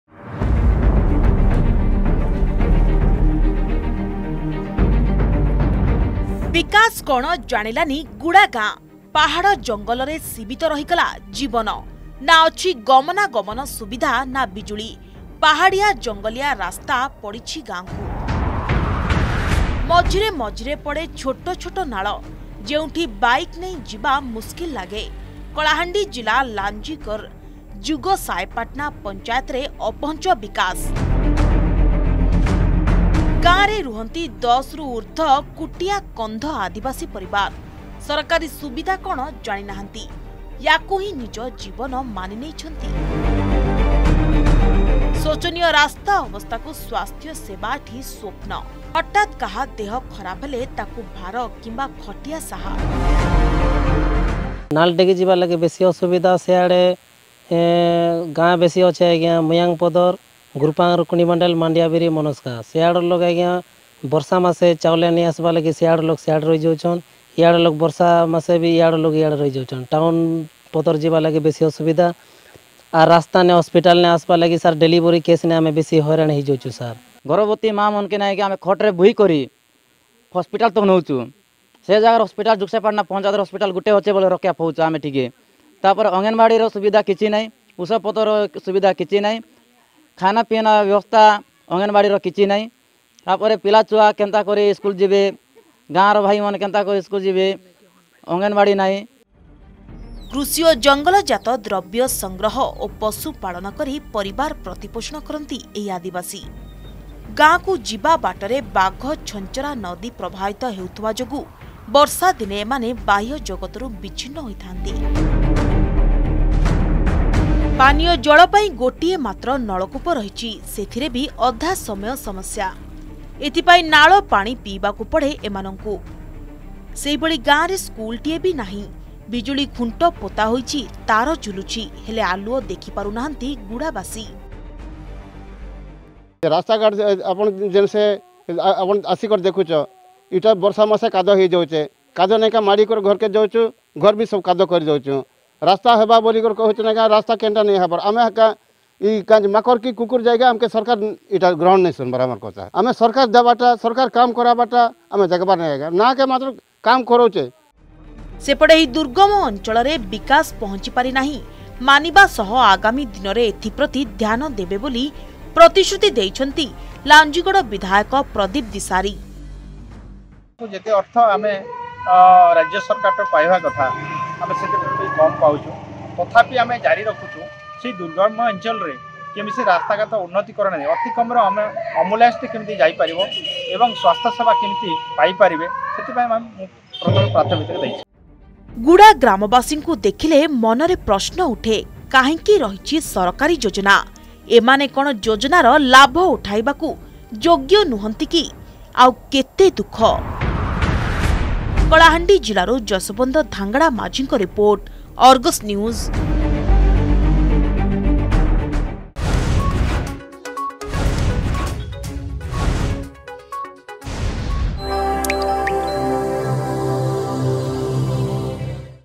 ବିକାଶ କଣ ଜାଣିଲାନି ଗୁଡା ଗାଁ ।
ବିକାଶ କଣ ଜାଣିଲାନି ଗୁଡା ଗାଁ । ପାହାଡ, ଜଙ୍ଗଲରେ ସୀମିତ ରହିଗଲା ଜୀବନ । ନା ଅଛି ଗମନାଗମନ ସୁବିଧା ନା ବିଜୁଳି । ପାହାଡିଆ, ଜଙ୍ଗଲିଆ ରାସ୍ତା ପଡିଛି ଗାଁକୁ । ମଝିରେ ମଝିରେ ପଡେ ଛୋଟ ଛୋଟ ନାଳ । ଯେଉଁଠି ବାଇକ୍ ନେଇ ଯିବା ମୁସ୍କିଲ୍ ଲାଗେ । କଳାହାଣ୍ଡି ଜିଲ୍ଲା ଲାଞ୍ଜିଗଜର ଜୁଗସାଏପାଟଣା ପଞ୍ଚାୟତରେ ଅପହଞ୍ଚ ବିକାଶ ।